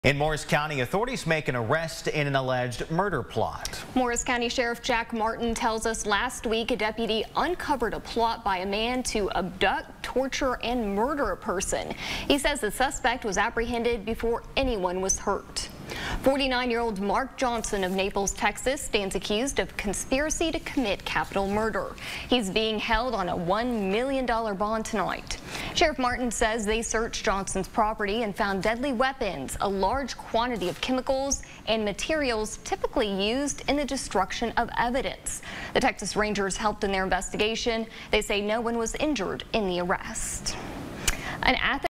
Report: Torture supplies found in investigation of Morris County murder plot
A man has been arrested for allegedly planning to abduct, torture and murder an individual in Morris County.